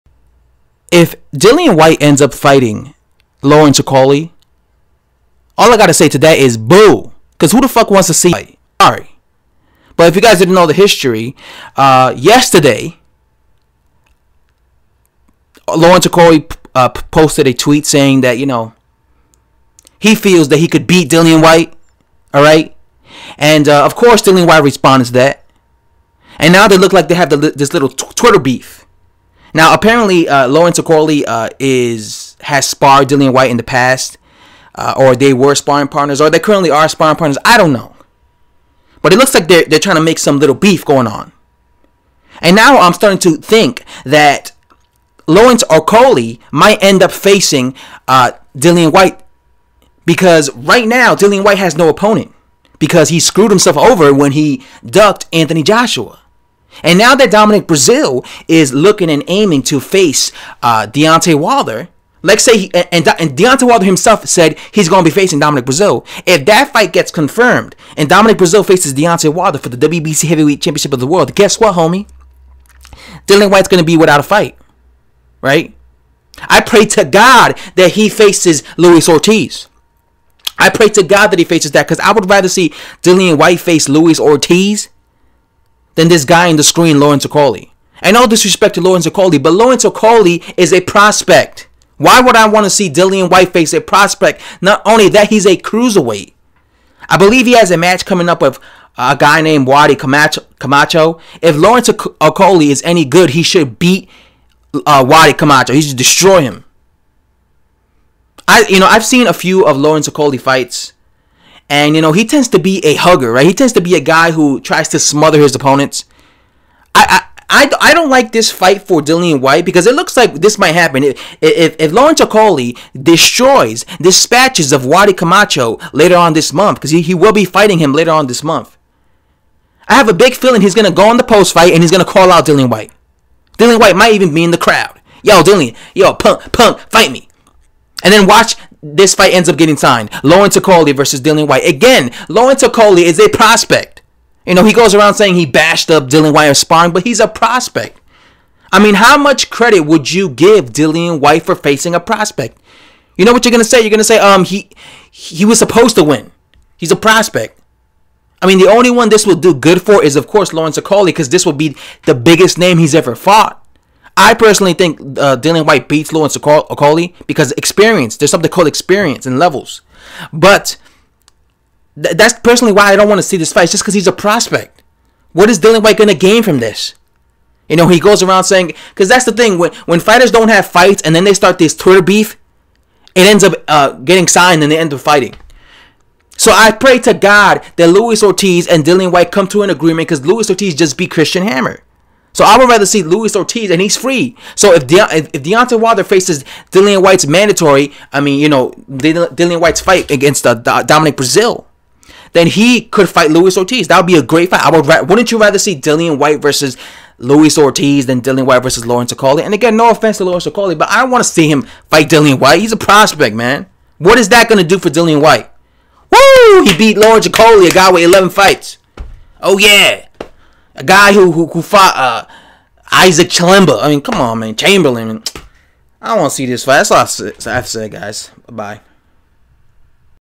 F#K NOO....DILLIAN WHYTE REDUCED TO FIGHTING NOVICE LAWRENCE OKOLIE FOR DUCKING JOSHUA!!!!
Looks like there might be a fight between Dllian Whyte and an 11-0 NOVICE Lawrence Okolie in the WORKS UGHHHH